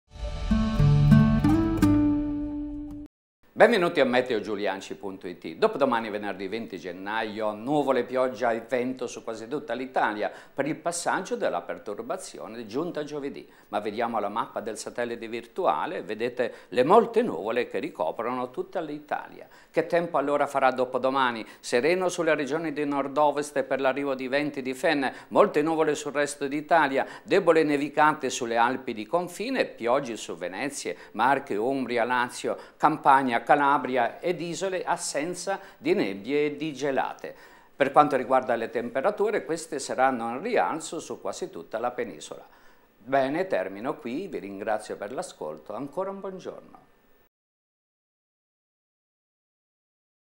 Meteo di dopodomani, venerdì 20 gennaio 2012.
Le previsioni del Colonnello Mario Giuliacci per dopodomani, venerdì 20 gennaio 2012.